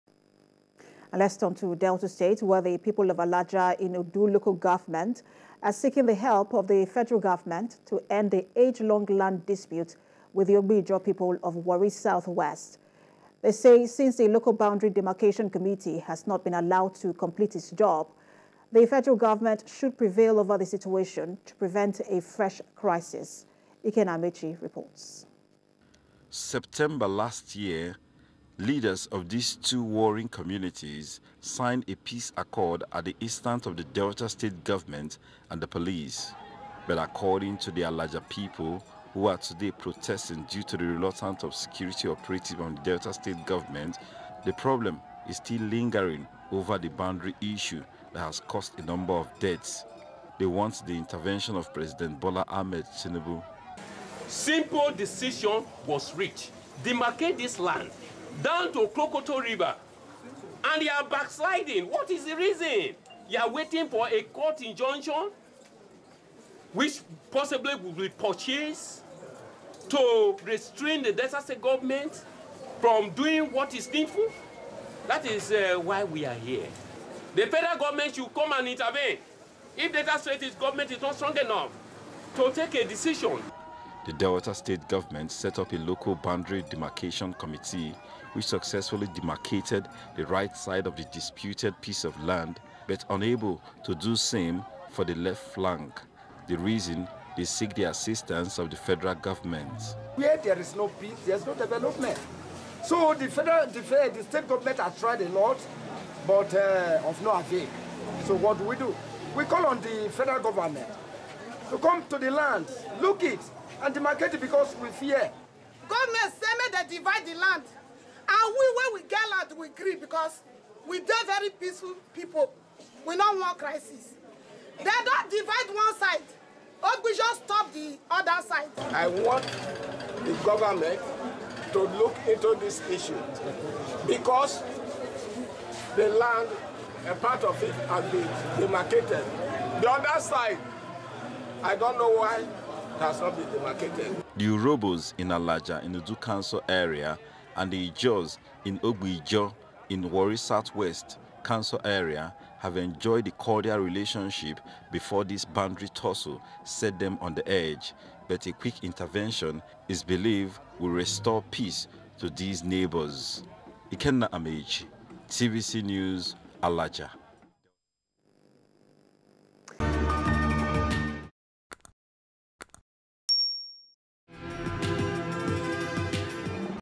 Aladja Community Protests Over Boundary Issues, Seeks Federal Government Assistance
The People of Aladja in Udu Local Government are seeking the help of the Federal Government to end the age-long land dispute with the Ogbe-Ijoh people of Warri South West.
They say since the local boundary demarcation committee has not been allowed to complete its job, the federal government should prevail over the situation to prevent a fresh crisis.